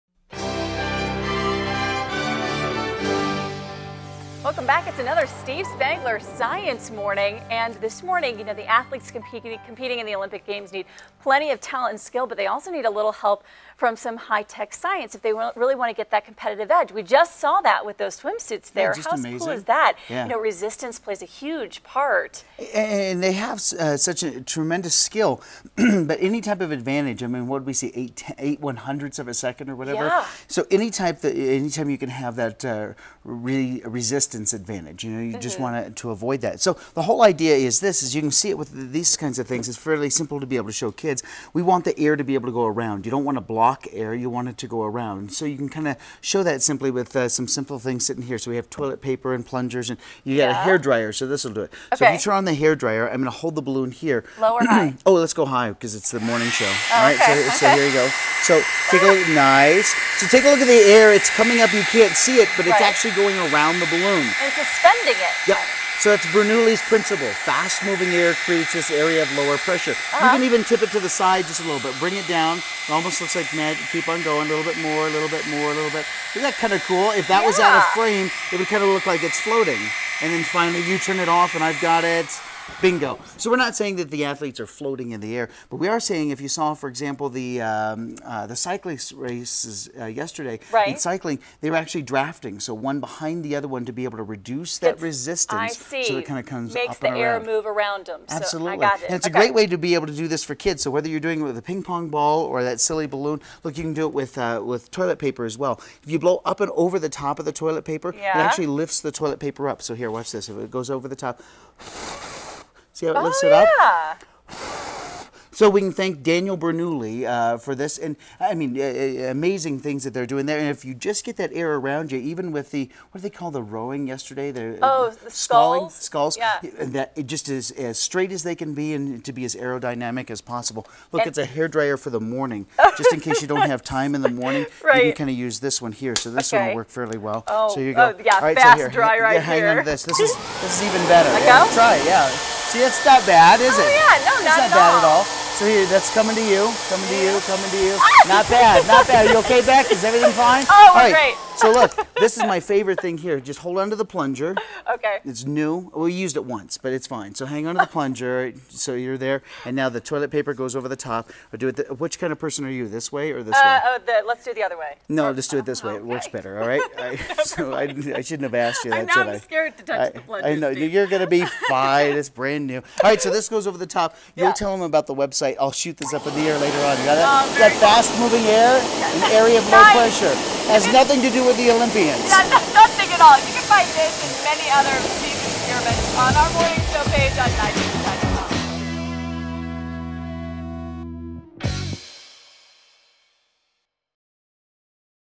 Science of the Olympics - Cool Science Experiments
Check out this and other cool science experiments I cant help but be amazed by the high-tech science that were seeing at this years Summer Olympics. The name of the game in so many of the sports is to reduce any opposing resistance. In some instances, the athlete can actually use other competitors to block the wind (drafting in cycling). What does any of this have to do with floating beach balls and flying toilet paper? That fast-moving air flowing around the object creates an area of lower pressure and gives the athlete an advantage. Thanks Bernoulli.